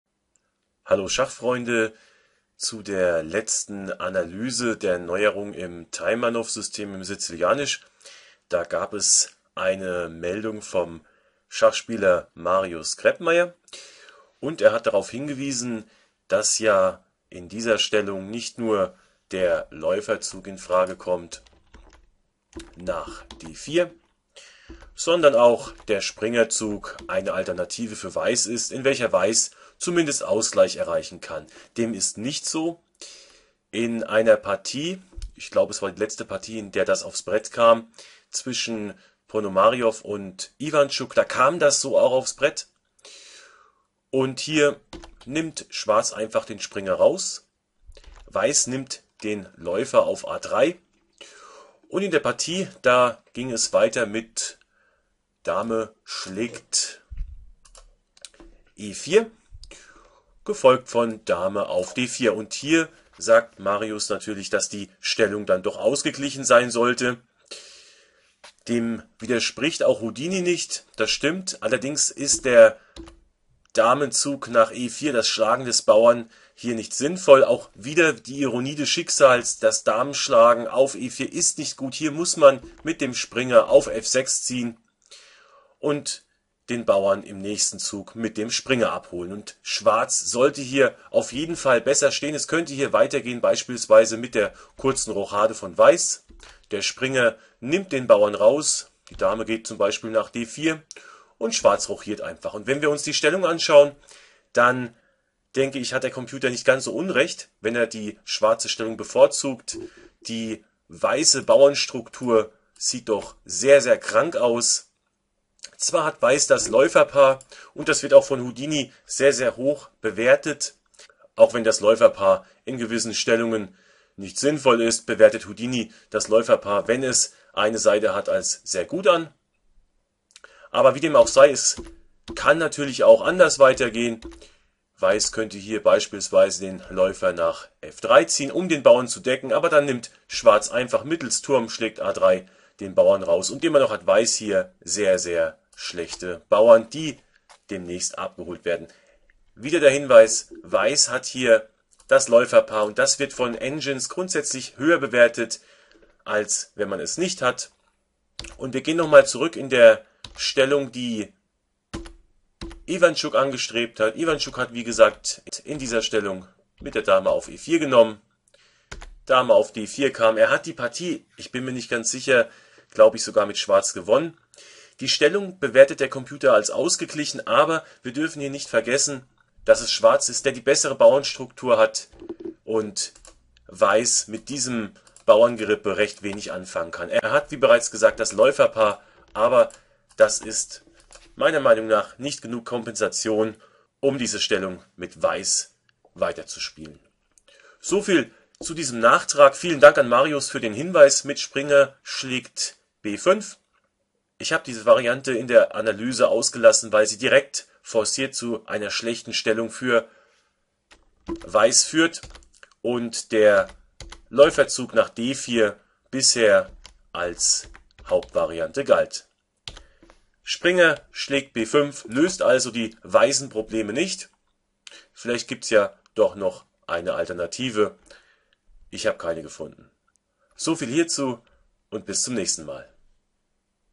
Schacheröffnung Sizilianisch Taimanov Neuerung - Eine Anmerkung zu Sxb5
Hier noch eine kurze Anmerkung zu der Idee Sxb5 nach La3 im Taimanov-System der Sizilianischen Verteidigung.